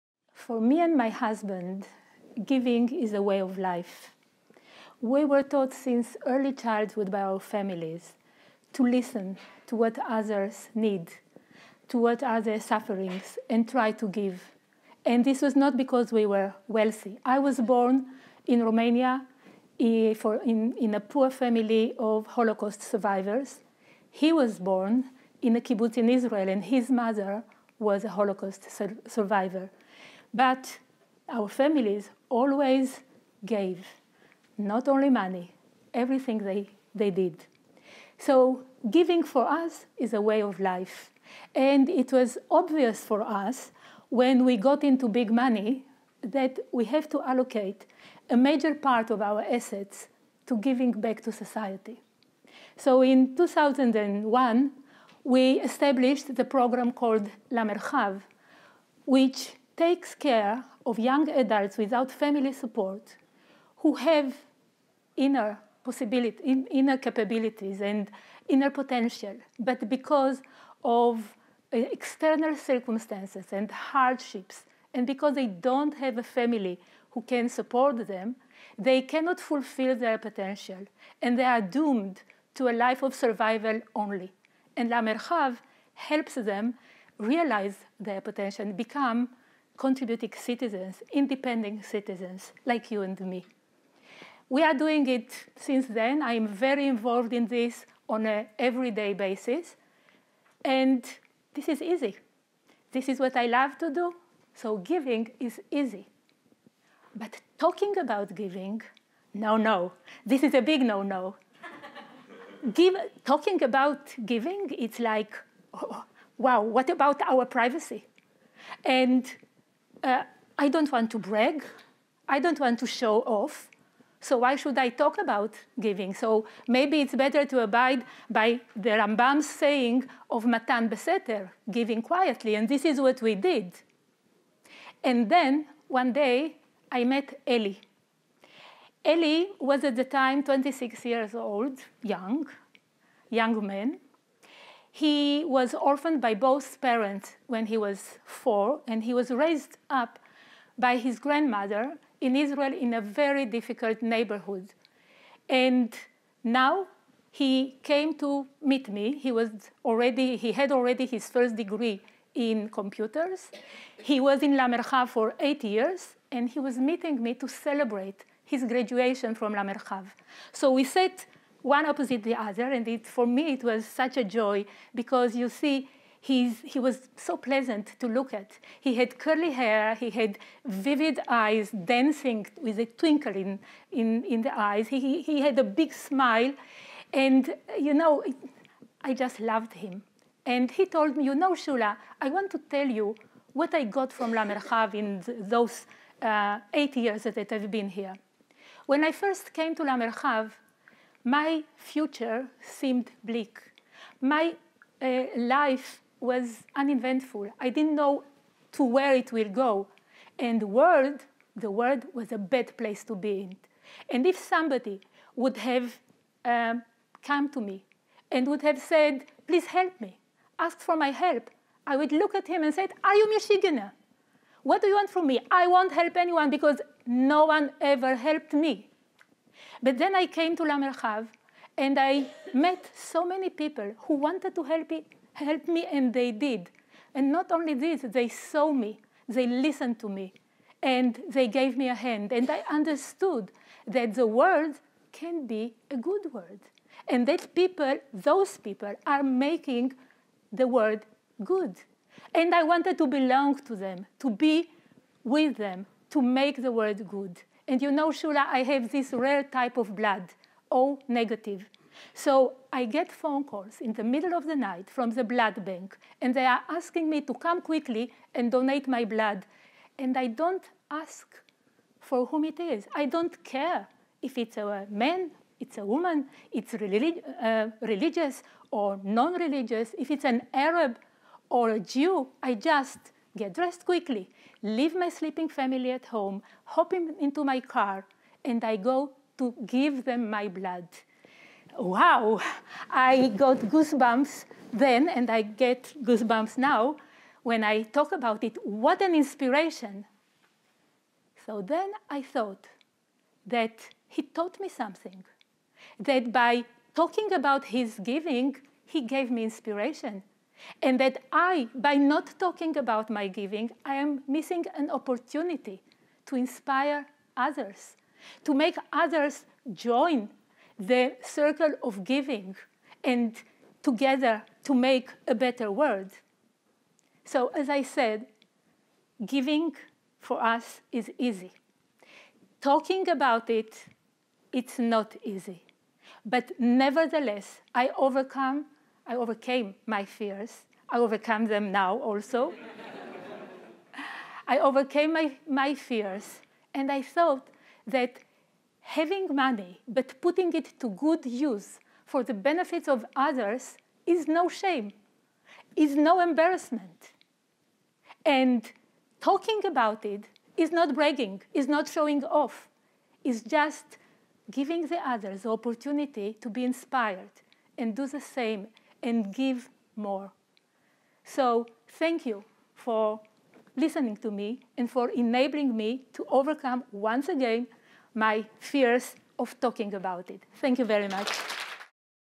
Shula Mozes: Giving is easy; talking about giving is hard.
Shula Mozes explains why giving is easy, why talking about giving is more uncomfortable, and why we have to talk about it anyway.